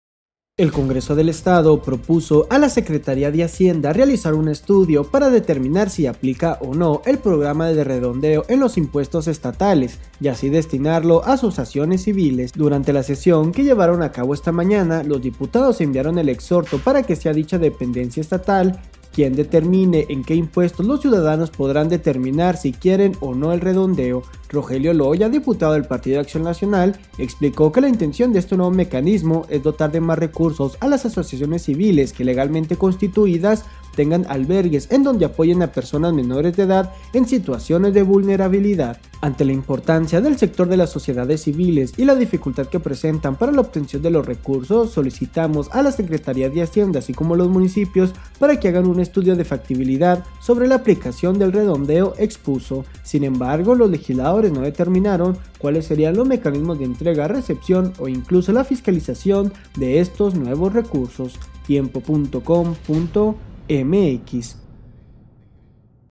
Tiempo TV | Quieren Diputados aplicar "redondeo" a impuestos estatales
El Congreso del Estado propuso a la Secretaría de Hacienda realizar estudio para determinar si aplica o no programa de "redondeo" en impuestos estatales y así destinarlo a asociaciones civiles.